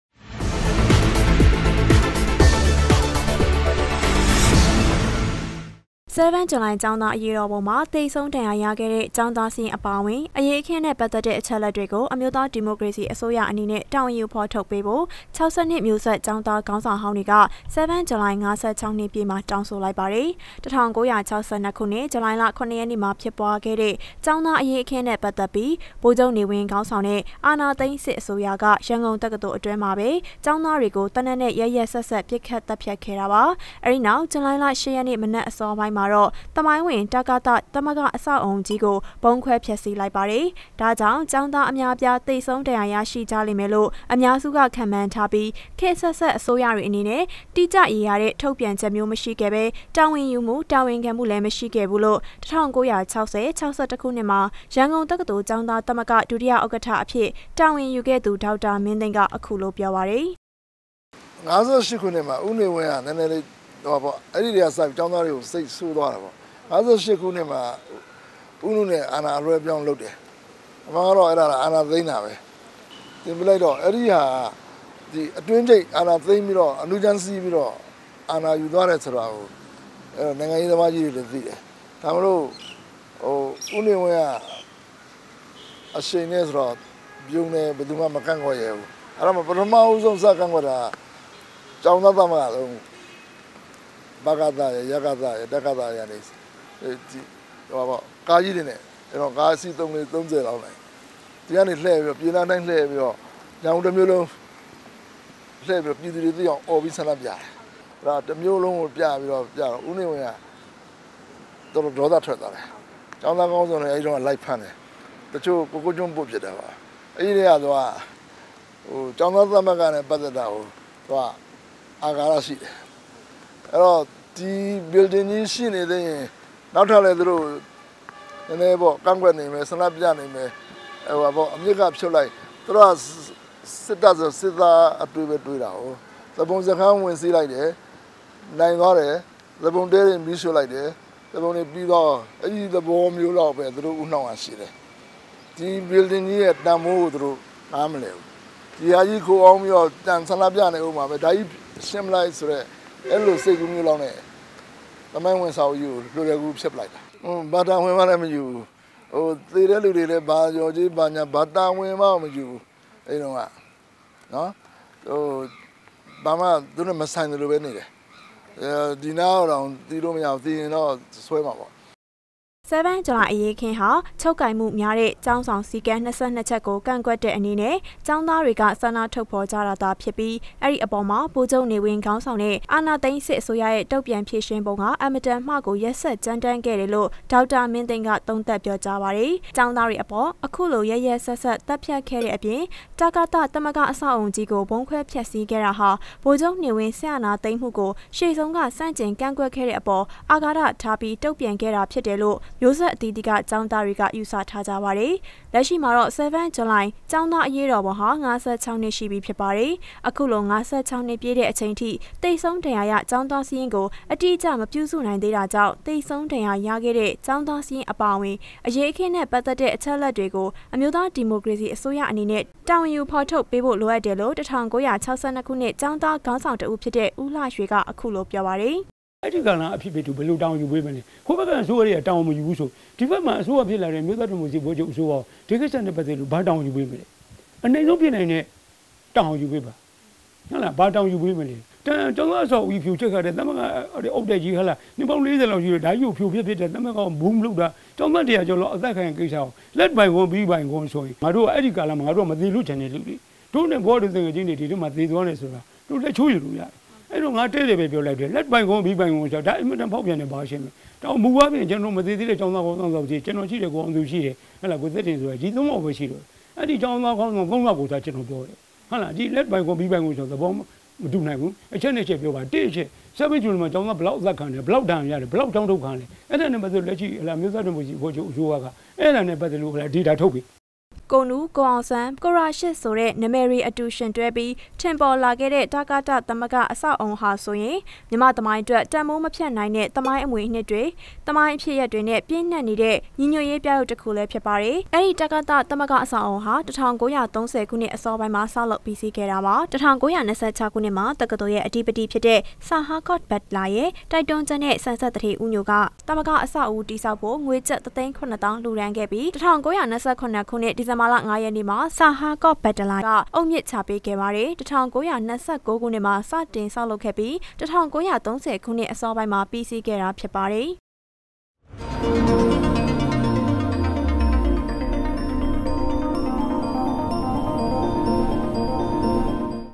7july ေက်ာင္းသားအေရးေတာ္ပံုတြင္ ေသဆံုး၊ ...
7july ေက်ာင္းသားအေရးေတာ္ပံုတြင္ ေသဆံုး၊ ဒဏ္ရာရေက်ာင္းသားစာရင္းအပါအ၀င္ အေရးေတာ္ပံုႏွင့္ သက္ဆိုင္သည့္ အခ်က္အလက္မ်ားကိုNLDအစိုးရအေနျဖင့္ေဖာ္ထုတ္ေပးရန္ေတာင္းဆို
 7 july ေက်ာင္းသားအေရးေတာ္ပံုမွာ ေသဆံုး၊ ဒဏ္ရာရခဲ့တဲ့ ေက်ာင္းသားစာရင္း အပါအ၀င္ အေရးအခင္း နဲ႕ ပတ္သက္တဲ့ အခ်က္အလက္ေတြကို အမ်ိဳးသားဒီမိုကေရစီအစိုးရအေနနဲ႕ တာ၀န္ယူ ေဖာ္ထုတ္ေပးဖို႕ ၆၂ မ်ိဳးဆက္ေက်ာင္းသားေခါင္း ေဆာင္ေဟာင္းေတြက ဆဲဗင္းဂ်ဴလိုင္ ၅၆ ႏွစ္ ျပည့္မွာ ေတာင္းဆို လိုက္ပါတယ္။